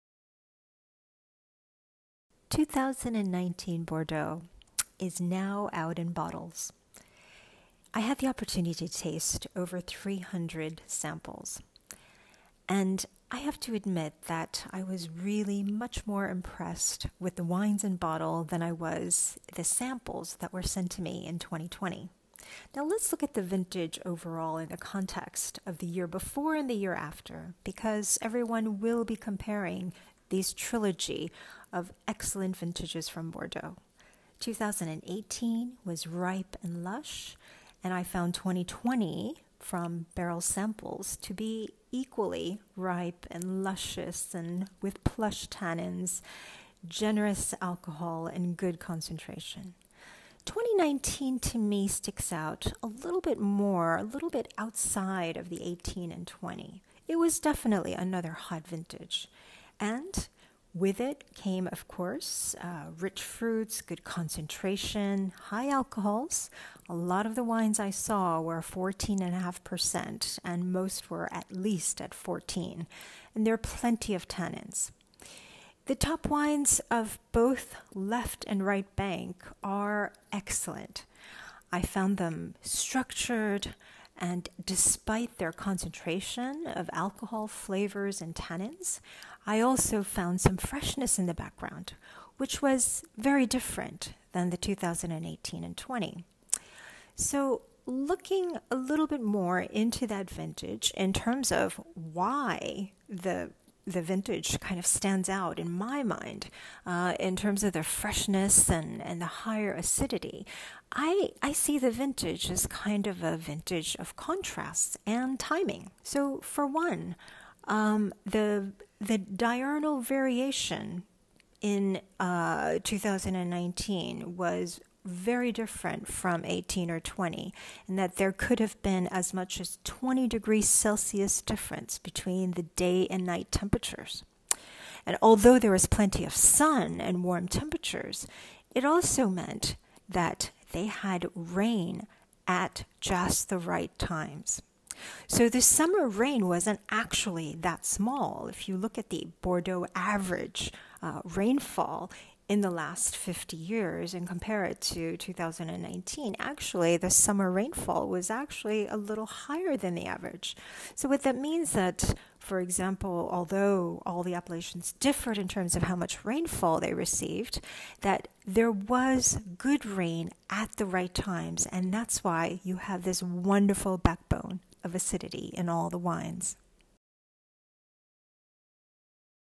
Discovering Wine with Jeannie│2019 Bordeaux in Bottle [Part 1]
Enjoy my 2-part overview of the top 2019 Bordeaux wines in bottle - find out why for me the 2019 stands out among the trilogy of warm & dry vintages (2018, 2019, 2020)!